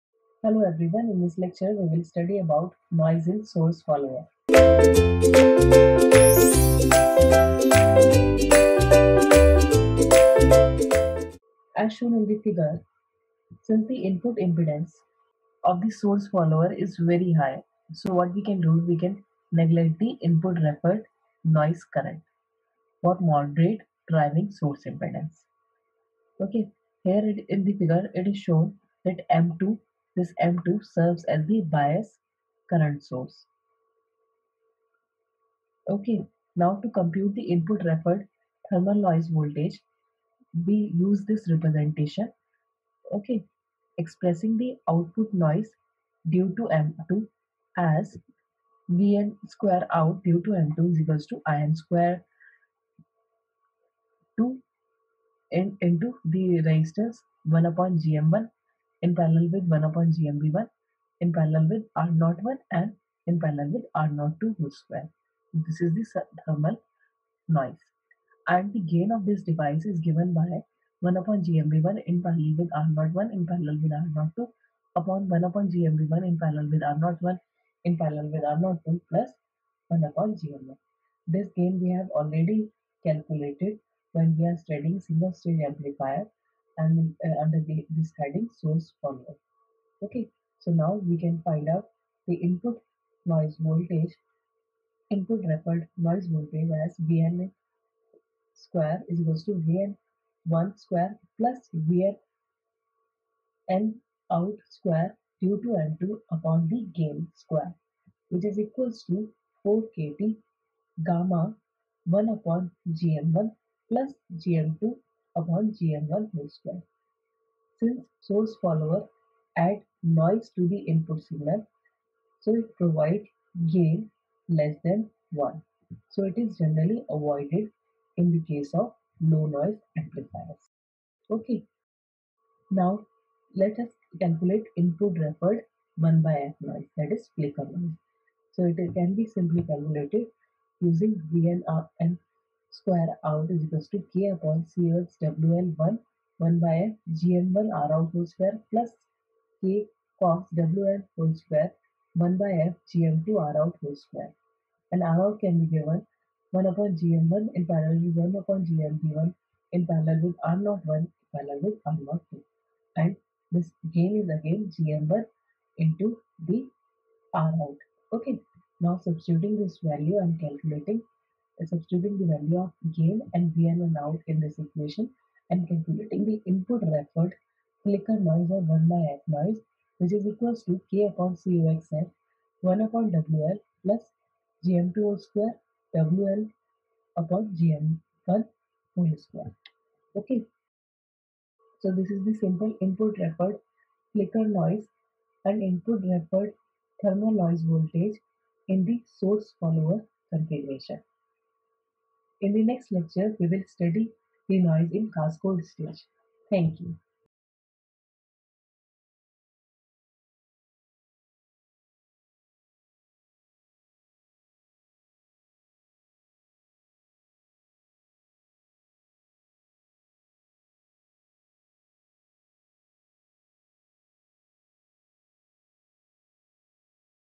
Noise in Source Follower - Amplifier Fundamentals - Analog & Mixed VLSI Design
Subject - Analog & Mixed VLSI Design

Topic - Noise in Source Follower

Chapter - Amplifier Fundamentals

Faculty - Prof. Radhika Goel

Happy Learning!